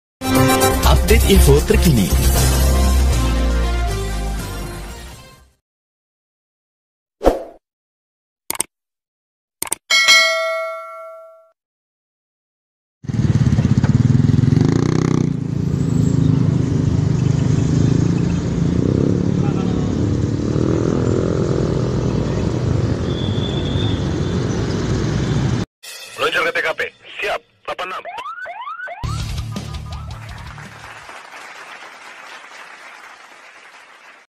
Kemacetan di Margonda ada evakuasi truk terguling depan UI - warkop goda gado
Kepadatan lalin di Margonda - Pasar Minggu, Jaksel sampai siang ini, karena ada evakuasi truk box terguling di depan UI, Jumat, 13/12/2019 pagi.

Info dari: @pii.yn

agar tidak ketinggalan update video² update terbaru